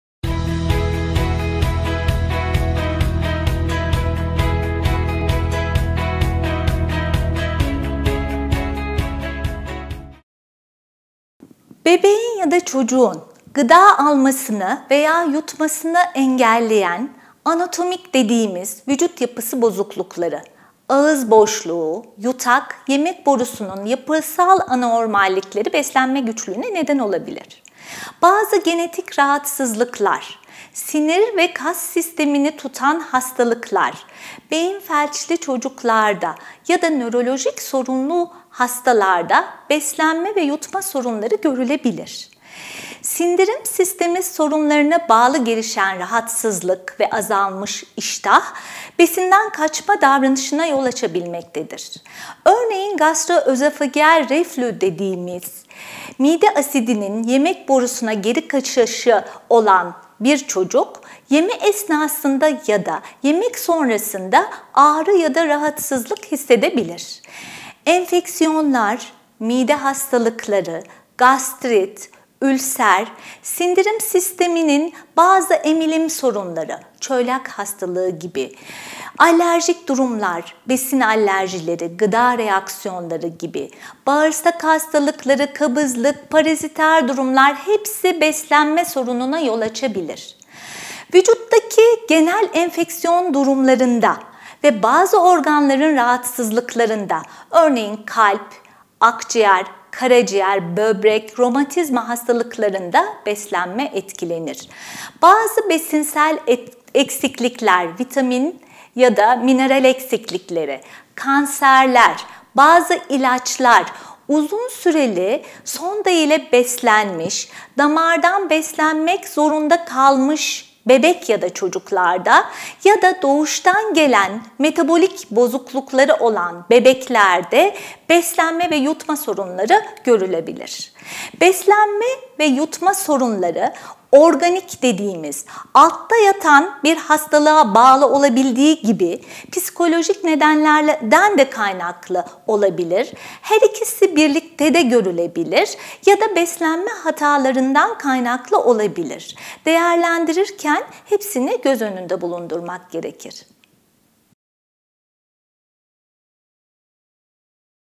Çocuklardaki beslenme ve yutma sorunlarının nedenleri nelerdir ?
Doç. Dr. Merve Usta "Çocuklardaki beslenme ve yutma sorunlarının nedenleri nelerdir ? " sorusunu cevaplıyor.